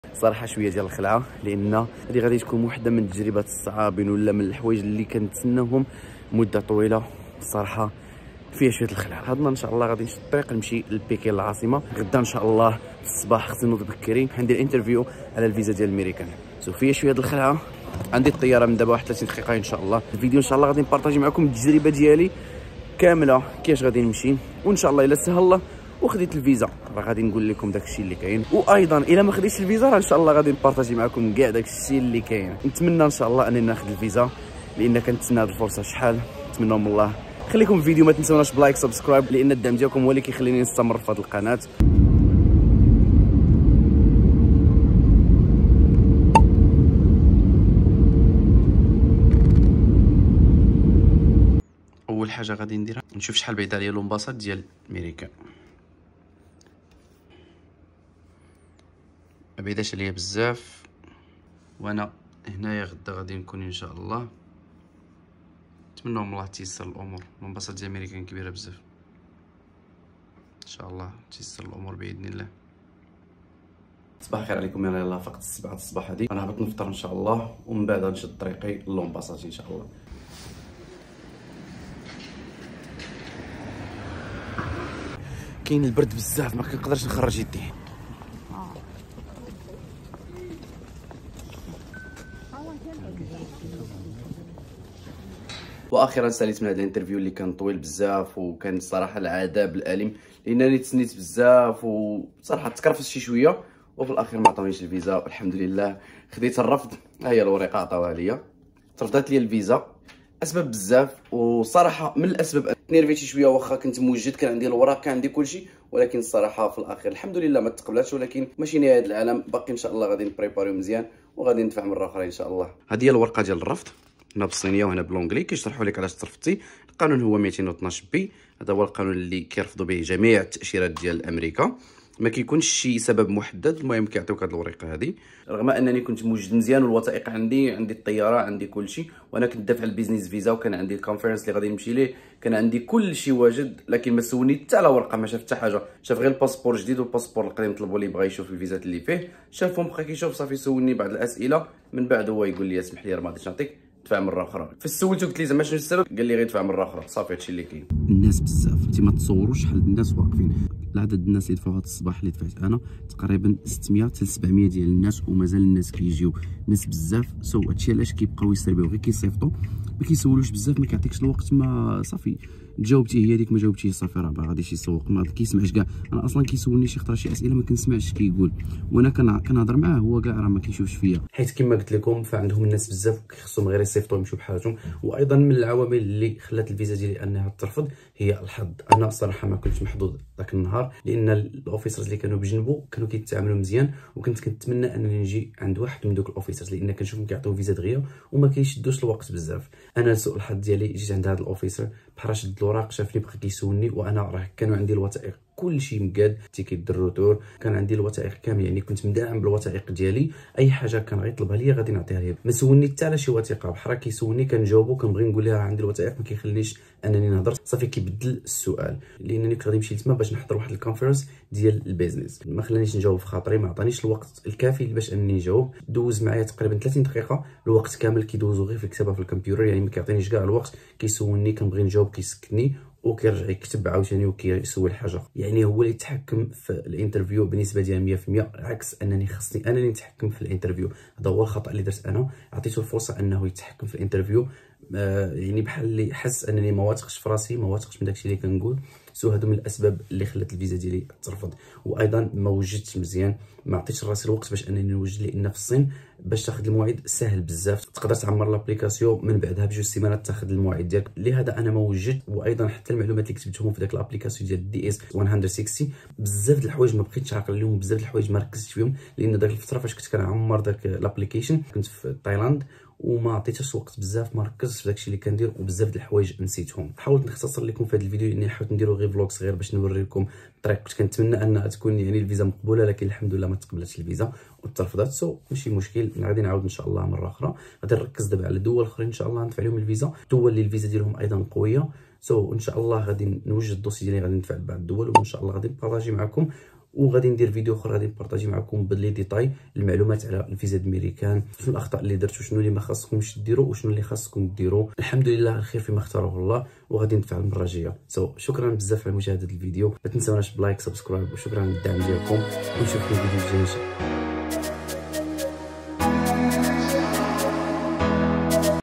لماذا تم رفض تأشيرتي الأميركية؟ اكتشف السر وراء ذلك الآن! 🤔 B1 Visa
لماذا تم رفض تأشيرتي الأميركية؟ اكتشف السر وراء ذلك الآن! 🤔 هل سبب رفض تأشيرتك الأميركية يشكل لغزاً بالنسبة لك؟ في هذا الفيديو، ستكتشف السر وراء رفض التأشيرة والطرق الفعّالة لتجاوز هذه المشكلة بنجاح. تعرف على الخطوات الحاسمة والاستراتيجيات المثبتة التي يمكن أن تساعدك على تحقيق نجاحك والحصول على تأشيرتك الأميركية بسهولة. تابع الفيديو الآن لاكتشاف المزيد!

تابعني على منصات التواصل الاجتماعي للمزيد من المغامرات والتحديات:
- [mouhssinearamchi] للانضمام إلى إنستغرام
- [maramchi92] للانضمام إلى Tiktok